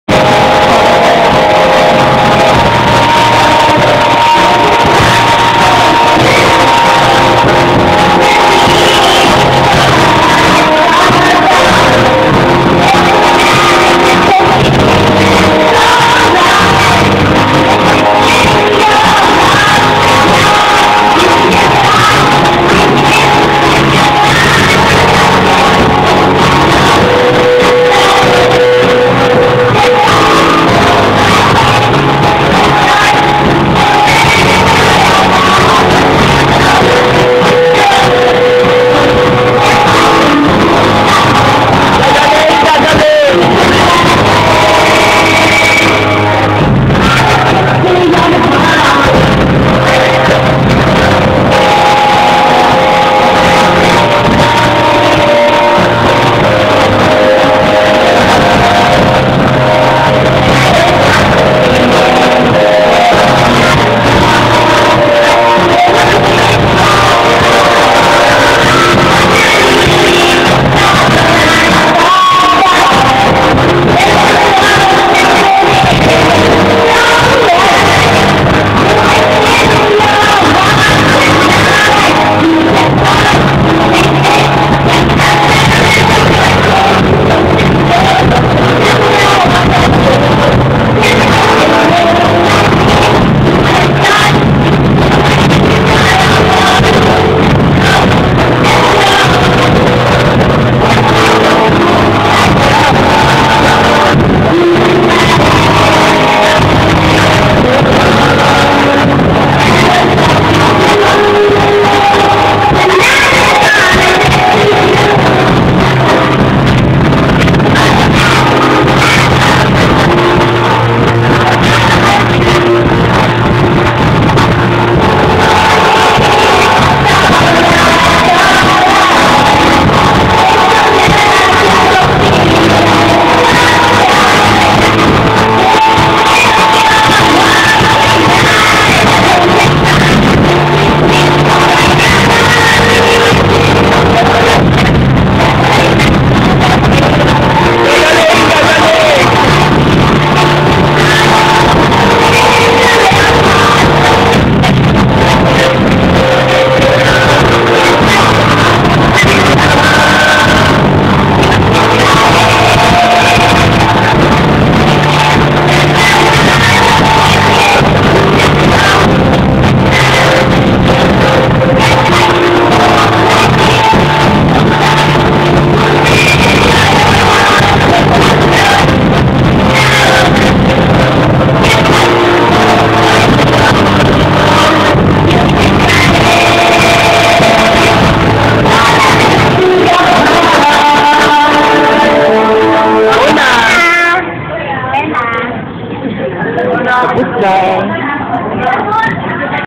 Team Albert's Drinking Session: Emelia on the House!!!!
our drinking session in Muchos Bar in Ortigas Center Pasig City. Medyo hindi maganda ang audio quality ng cellphone ko. Ngayon ko lang nalaman, pagkatapos kong i-review ung mga videos.